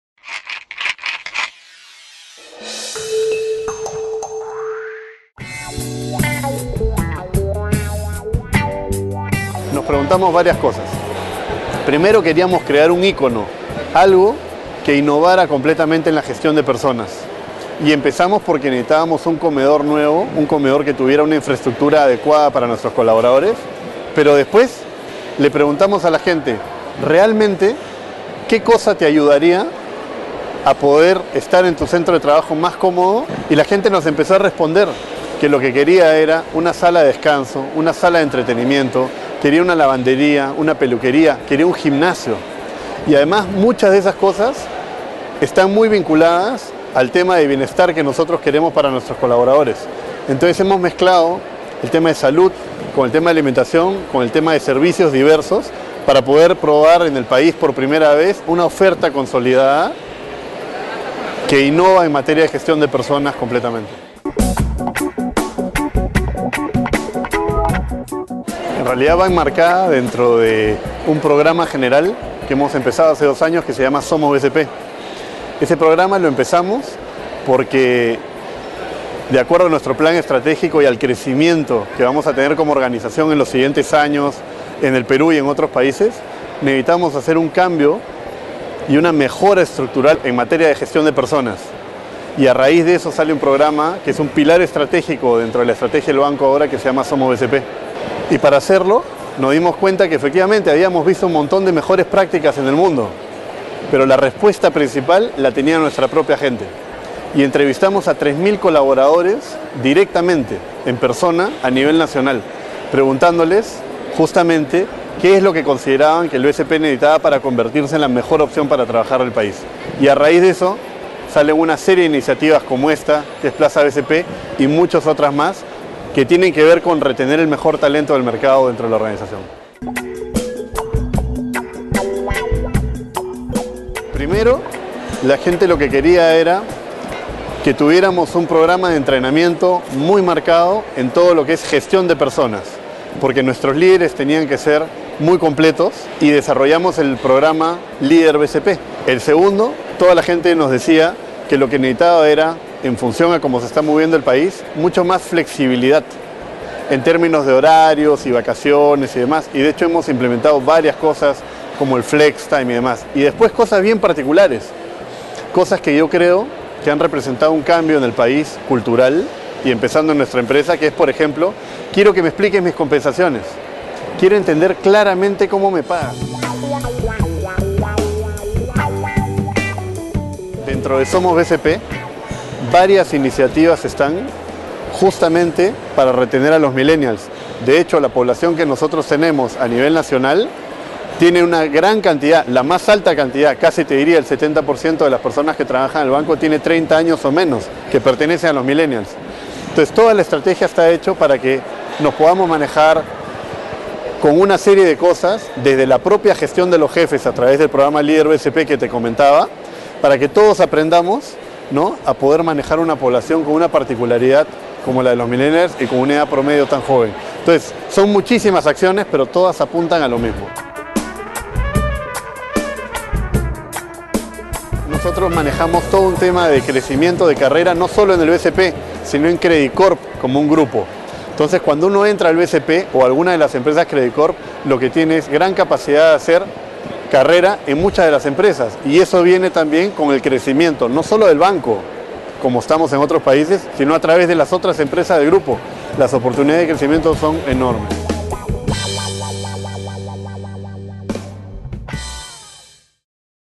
Plaza BCP: Donde Entretenerse y Trabajar Es Parte de una Estrategia para Retener el Talento
Gestión 30OCT'13

¿Se imagina un gimnasio, una peluquería, una lavandería y una sala de billar en su trabajo? No es un sueño: la tendencia de oficinas al 'estilo Google' llega al Perú y responde a las expectativas de los millennials. El banco apuesta por ello.

¿Jugar billar con sus colegas, acudir a la peluquería o lavandería por unos momentos, todo ello, en su centro de trabajo, sin la oposición de su jefe? La tendencia de generar espacios de ocio en la propia oficina ha cobrado popularidad en Estados Unidos y Europa. De hecho, multinacionales como Google son también conocidas por contar con lugares de recreación para sus empleados.

Ese concepto ha llegado a nuestro país de la mano del Banco de Crédito. Dentro de una estrategia integral que apunta a reclutar y retener talento, “la iniciativa Centro de Bienestar BCP se enmarca dentro del programa Somos BCP, que busca convertir al banco en la mejor opción para trabajar del país”.

Estas fueron las primeras reacciones de sus trabajadores tras la inauguración.